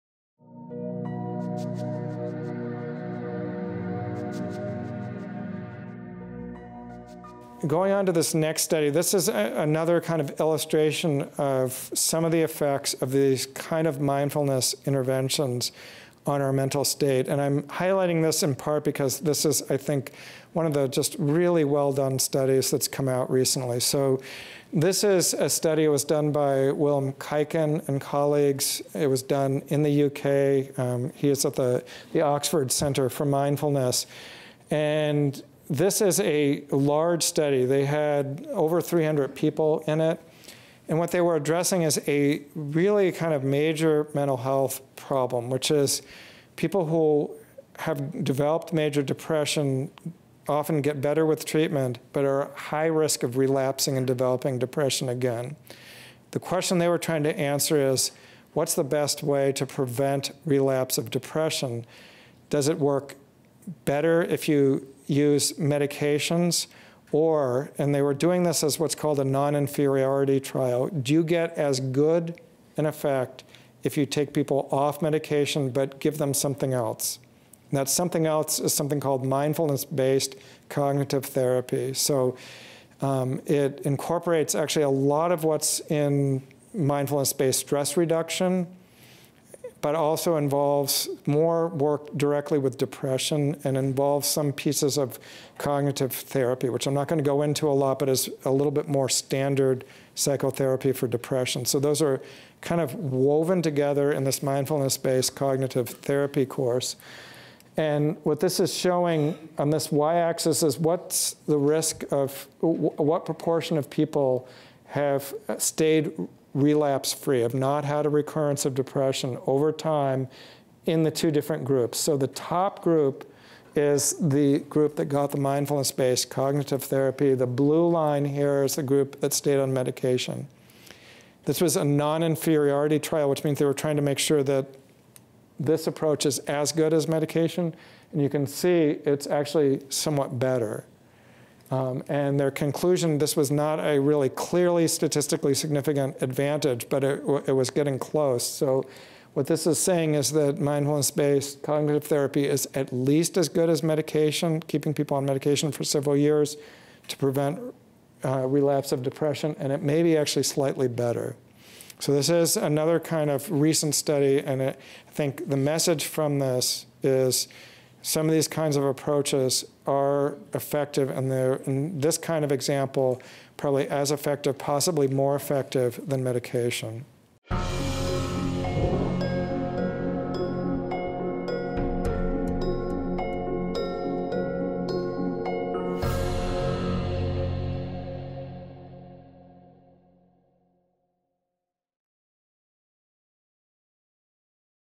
Treating Depression with Meditation
Dr. Rick Hecht shows the statistical evidence that seems to demonstrate those who suffer from depression may be less likely to relapse into a depressed state when practicing mindfulness based cognitive therapy as compared to those using medication. [Show ID: 31312]